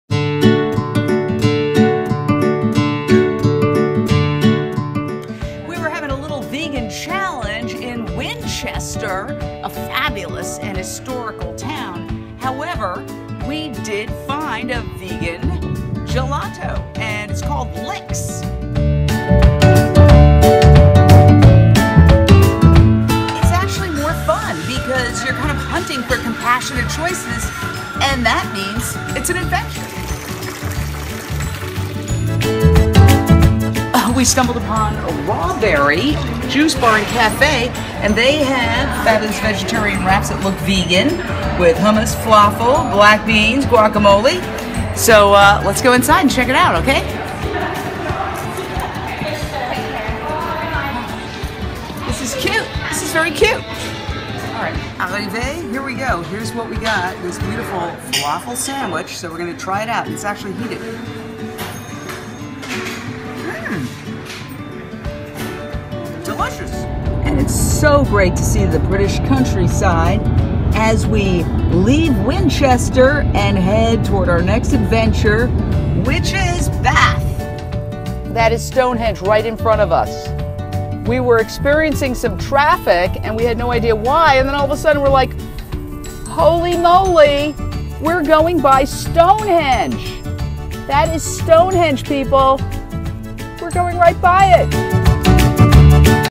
Episode 2: Vegan England. Finding Vegan Options in Winchester, England
Winchester is the historic home of the famed Winchester Cathedral and legendary King Arthur's Court & Knights of the Roundtable. A magical place, filled with history, it is - alas - not chock full of vegan options. YET WE FOUND SOME! Yay! That's the adventure of veganism! WATCH!